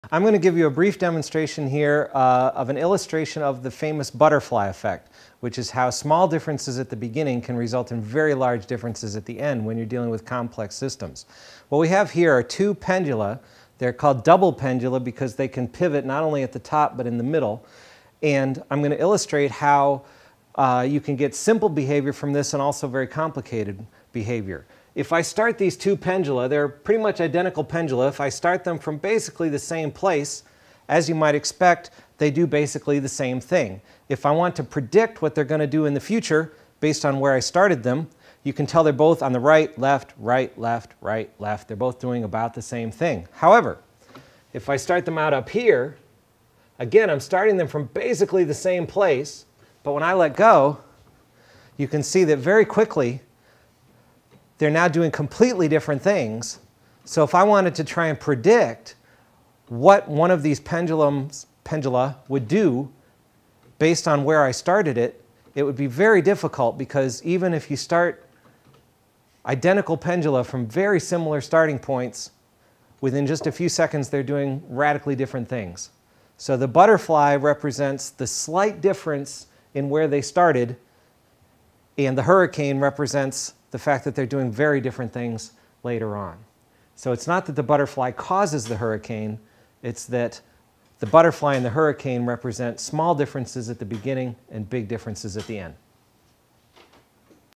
The "Butterfly Effect" Explained | Guilford College
Guilford College Assistant Professor of Physics Don Smith explains the "butterfly effect."